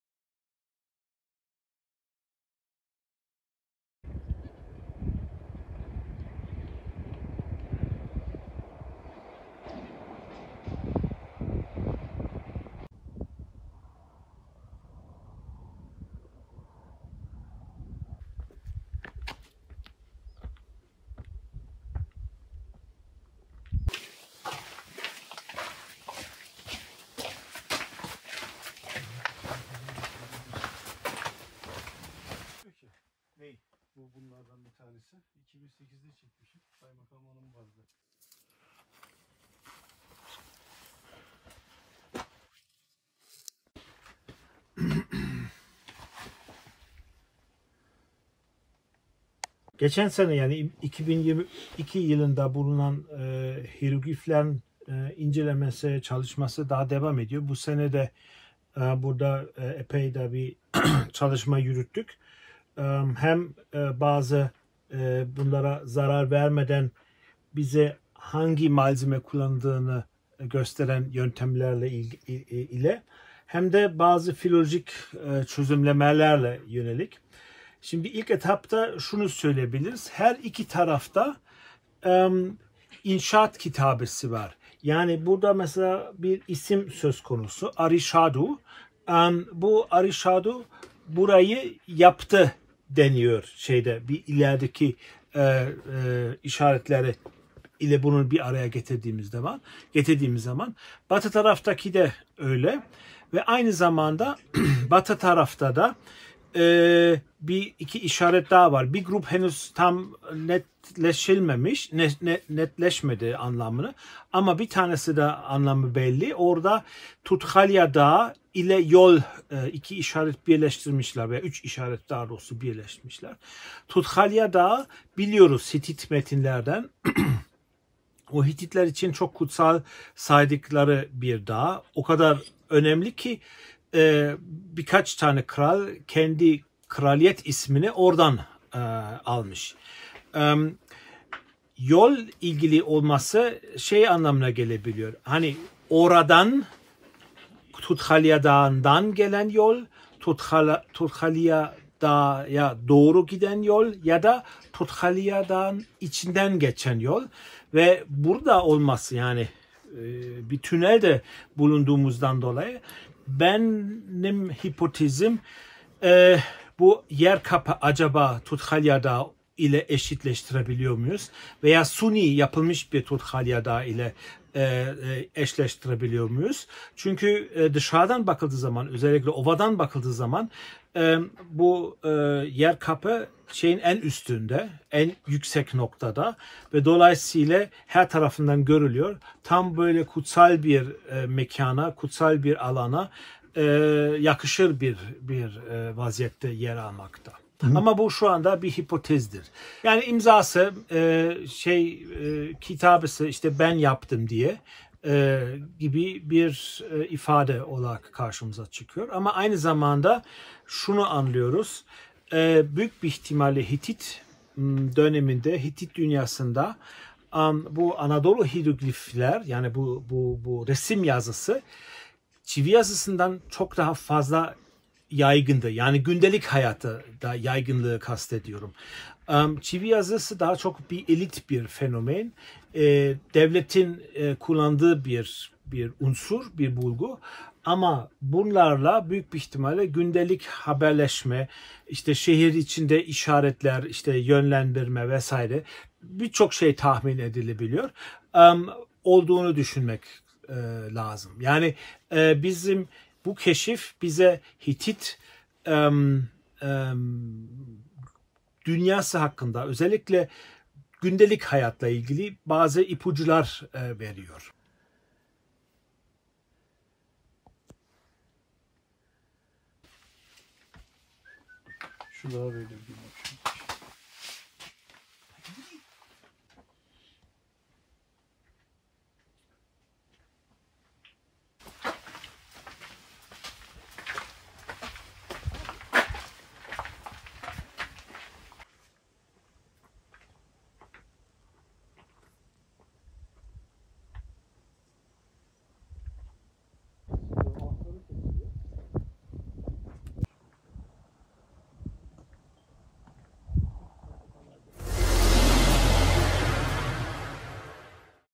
Hattuşa Yerkapı Tüneli'nde bulunan hiyeroglifler
Hititlerin başkenti Hattuşa'daki Yerkapı Tüneli'nde geçen yıl keşfedilen Anadolu hiyerogliflerinin (Yaklaşık 500 işaretten oluşan bir logografik yazı biçimi) çözülen bölümlerinden bir kısmı, tüneli yapan usta hakkında bilgi veriyor...

Anadolu'nun ilk medeniyetlerinden Hititlerin başkenti, Çorum'un Boğazkale ilçesindeki Hattuşa Ören Yeri'nde 80 metre uzunluğunda, binlerce taş kullanılarak inşa edilen Yerkapı Tüneli'nde, Mardin Artuklu Üniversitesi Arkeoloji Bölümü Öğretim Üyesi Doç. Dr. Bülent Genç tarafından 13 Ağustos 2022'de bulunan hiyerogliflerin farklı uzmanlık alanlarındaki bilim insanlarınca incelemesi sürüyor.
Yaklaşık 3 bin 300 yıllık olabileceği tahmin edilen, bir çeşit boya kullanılarak çizilmiş 249 simgenin, tünelin her mevsim ortalama sıcaklığa sahip olması, güneş ve yağmur girmeyen karanlık bir ortamda bulunması gibi koşullar sayesinde binlerce yıldır bozulmadan bugüne ulaştığı değerlendiriliyor.
Hattuşa Kazı Başkanı, Alman Arkeoloji Enstitüsünden Prof. Dr. Andreas Schachner, simgelerin yapımında kullanılan malzemeler ve dil bilimi açısından yürütülen çalışmalarda, 249 işareti altı gruba ayırdıklarını söyledi.
Kazı Başkanı Prof. Dr. Andreas Schachner: "Buradaki bir simgede 'Arişadu' şeklinde bir isim söz konusu. Bu ve ilerideki işaretleri bir araya getirdiğimizde 'Burayı Arişadu yaptı' anlamı çıkıyor"